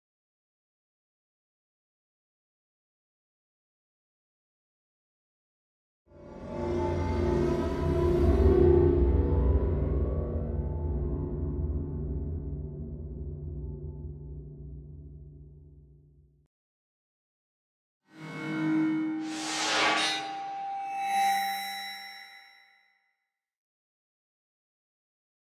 Matrix Text Effect created with Apple Motion
The "Matrix text effect" --

(Thought I'd take a little break from 3D modeling — turns out, this is worse with respect to rendering).

Basically showing it can be done in Motion, but like the opener says: a 10 second generator takes 30+ minutes to render... :P .
If it hadn't bogged down so badly, I would have gotten closer to the "real thing".

I may write about it but I'd like to figure out how to make it more user friendly first!